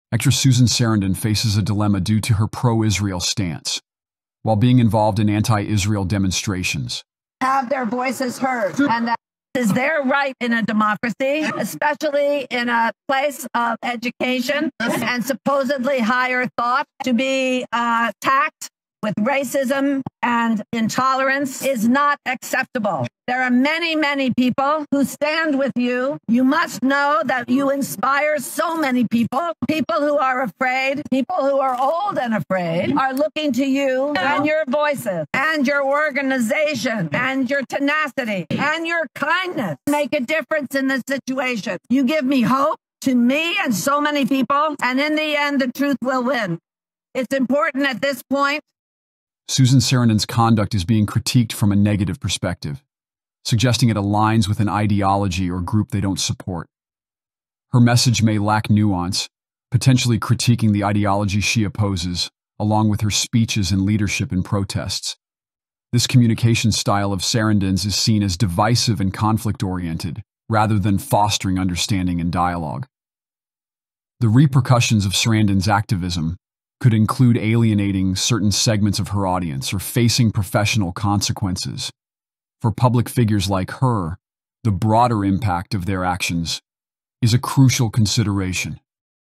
Actress Susan Sarandon, Joins Columbia University Protests
In this video, we delve into the recent controversy surrounding Hollywood actress Susan Sarandon's participation in anti-Israel protests at Columbia University. Susan Sarandon's bold stance and the reactions to her actions are explored, shedding light on a heated topic of debate.